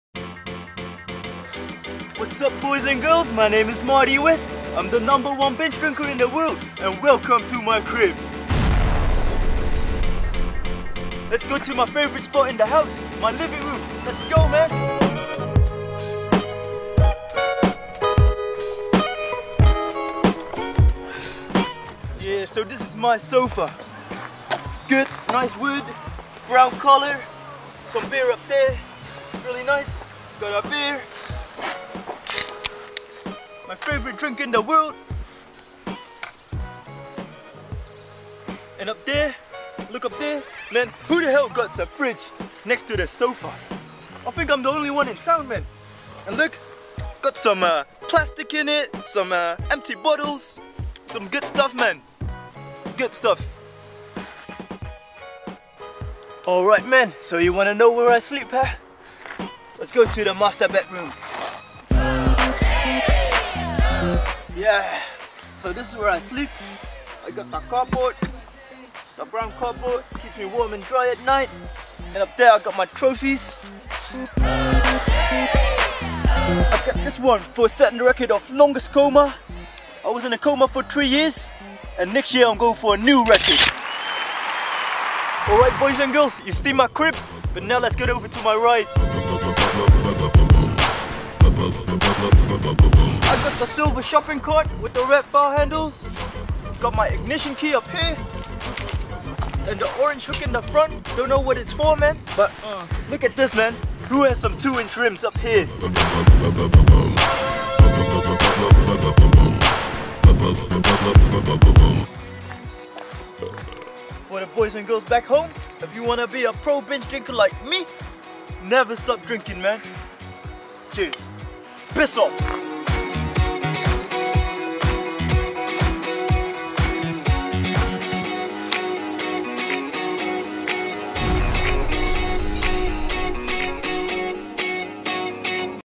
Marty West: The Greatest Pro Bingedrinker in The World
Marty grew up on an island where the water is clear and where the sun shines bright! Marty was born as the son of a carpenter and a virgin! At the age of 16 his skinny ass was discovered as a American Football talent, as a Quarterback, Marty soon found himself playing in various College Football teams in the States! He also attended Harvard university, (he actually was pretty smart) where he was following a course to be a succesfull, hard working, money making lawyer. Only one month before graduation (Springbreak) he discovered the wonderfull world of alcohol and gave everything up to persue his dream of being a PRO BINGEDRINKER!